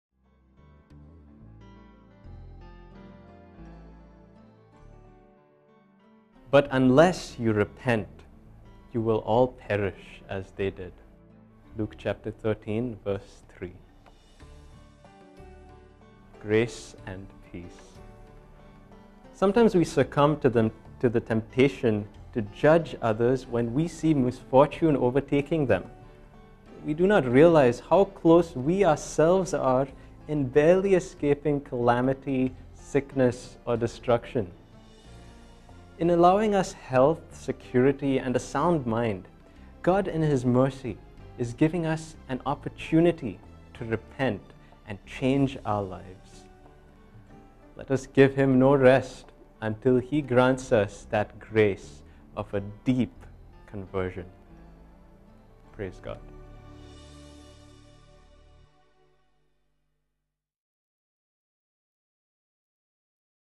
Daily Reflection 10/09/2013
Instead of judging others, look at your own heart and repent